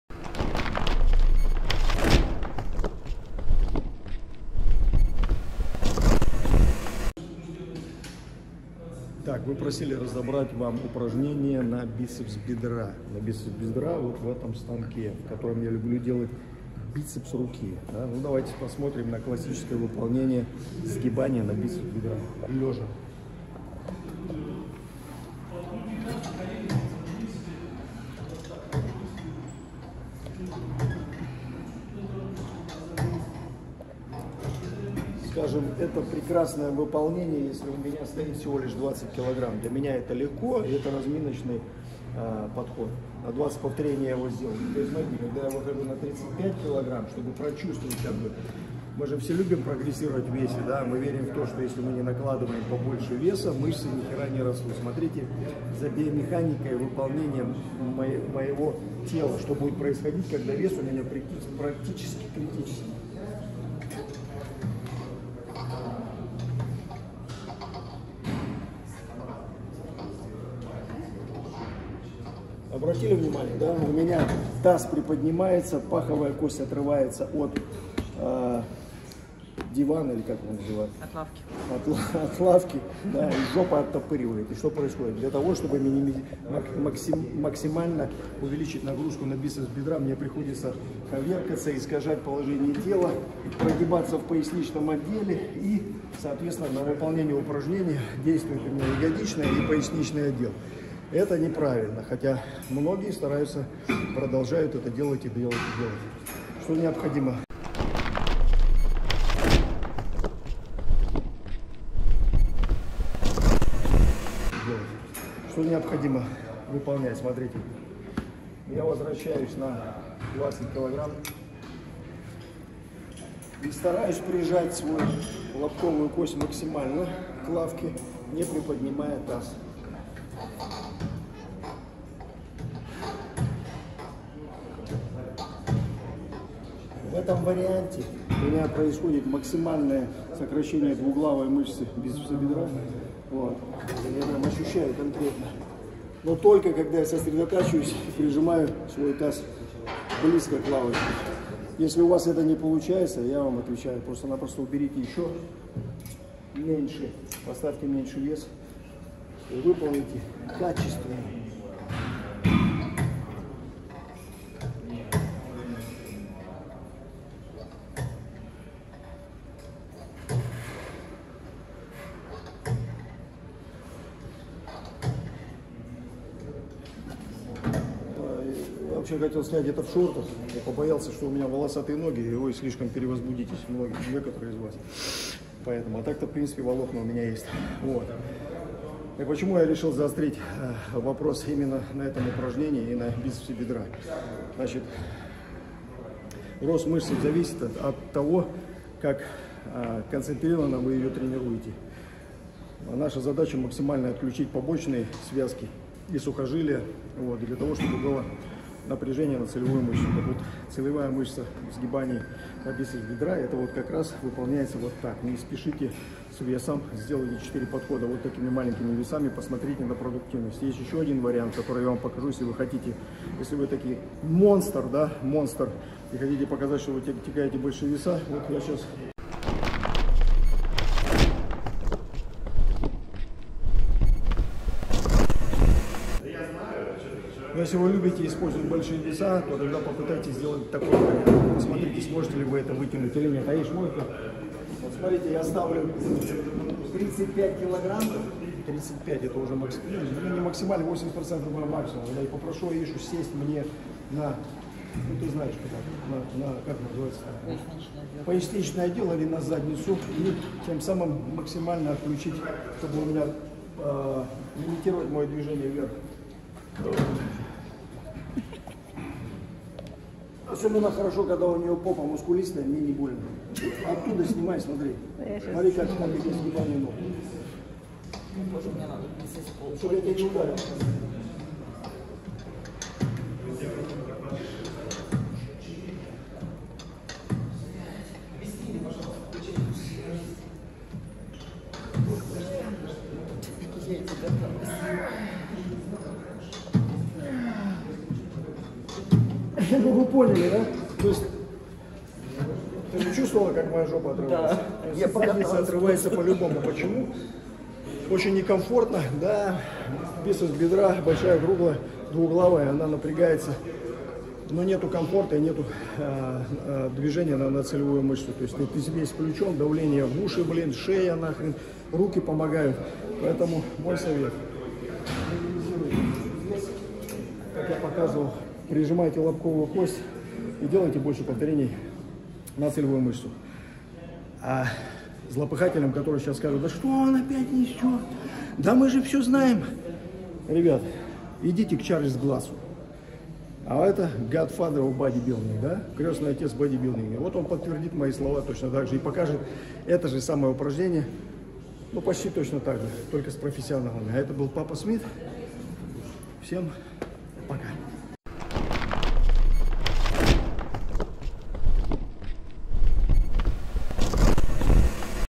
Правильная техника на бицепс бедра в тренажёре лёжа.
Мастер спорта по бодибилдингу, силовой и кондиционный тренер олимпийской сборной Китая, автор книги «МНОГОПОВТОРКА ДЛЯ ЧАЙНИКОВ», персональный тренер Владислав Кузнецов ака Папа Смит показывает правильную технику сгибания на бицепс бедра в тренажёре лёжа. Делайте качественно, и не гонитесь за весом.